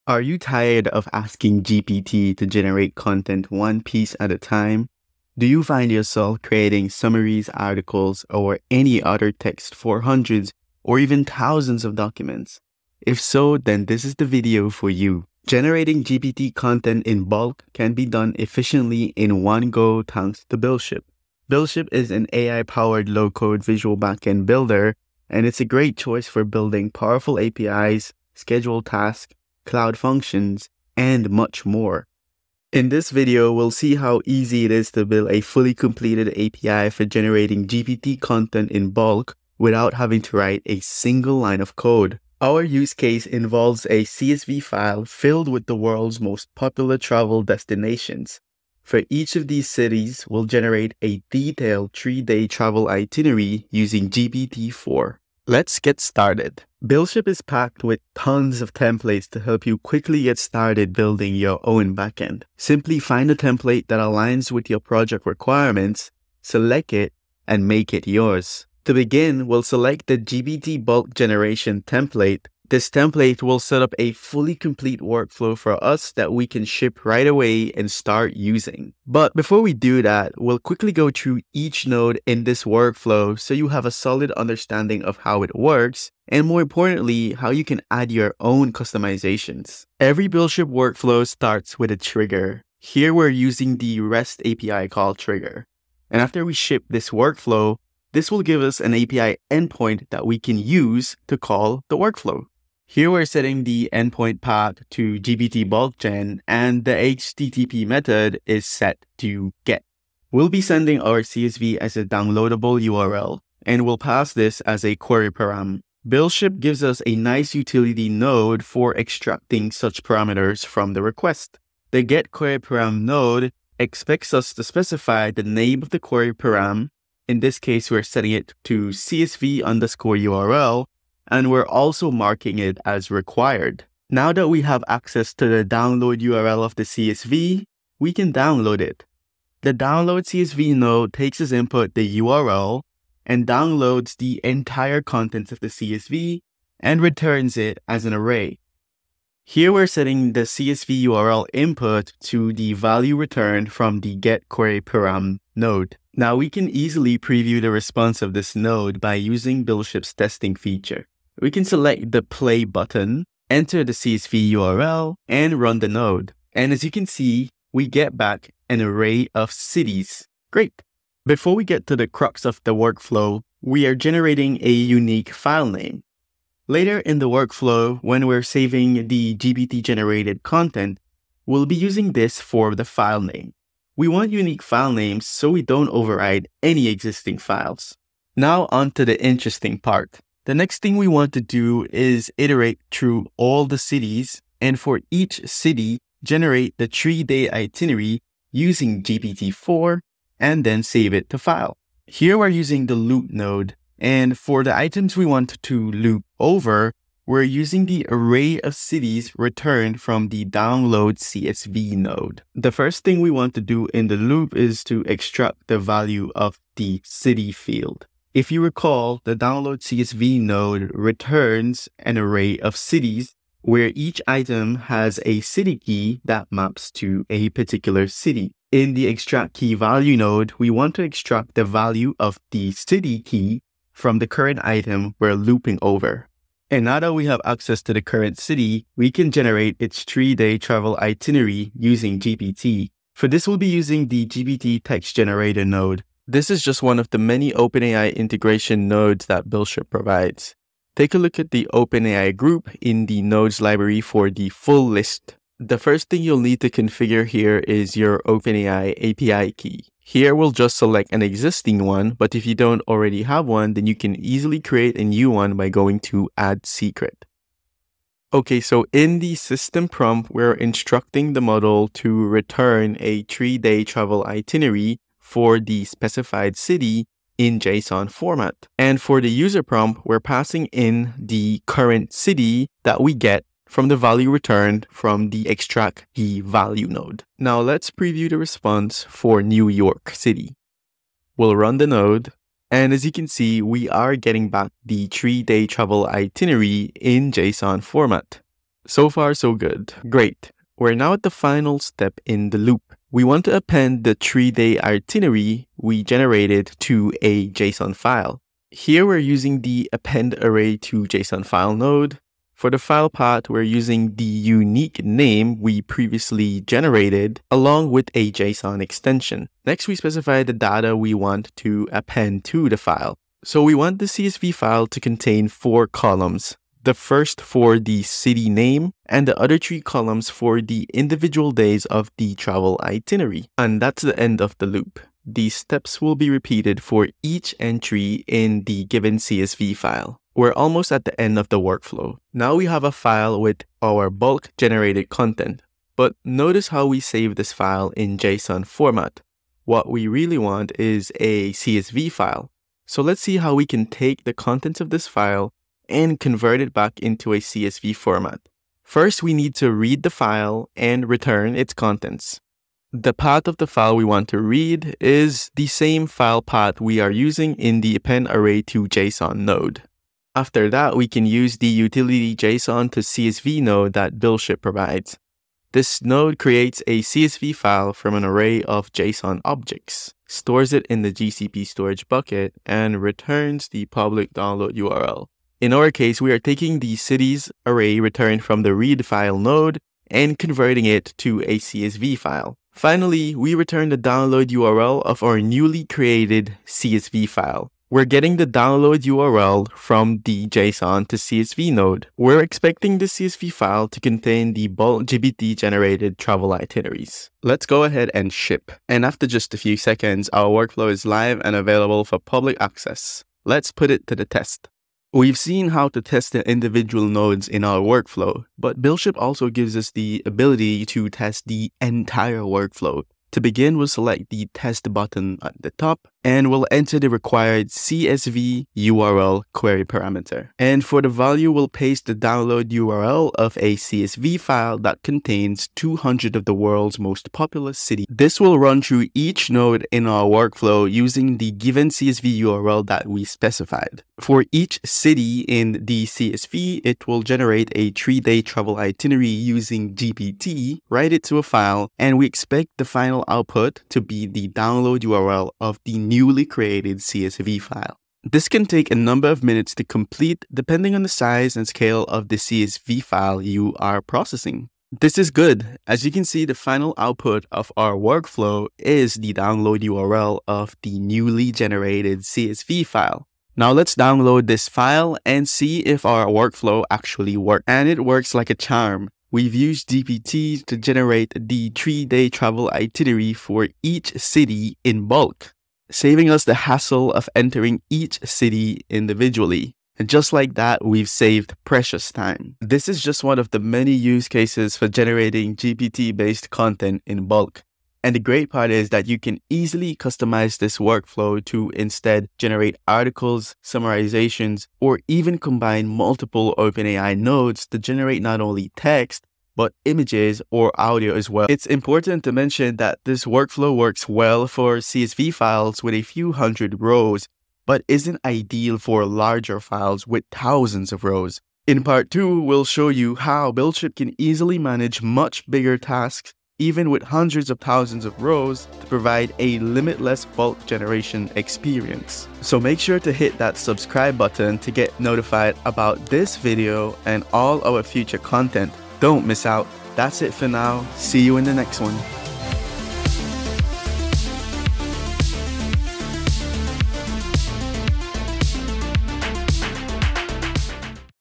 Bulk GPT - Run OpenAI API on a CSV file in parallel for every row
Learn how to run openAI in parallel on every line of a CSV file in bulk

A Low-code Visual Backend Builder with AI

Automate Bulk GPT Content Generation with BuildShop

00:00 Introduction to Bulk Content Generation with GPT
00:32 Getting Started with BuildShip for API Creation
01:30 Building the Workflow: From Trigger to Data Extraction
03:11 Generating Itineraries: The Core Workflow Process
05:45 Finalizing and Testing the Workflow
07:51 Results, Customization, and Conclusion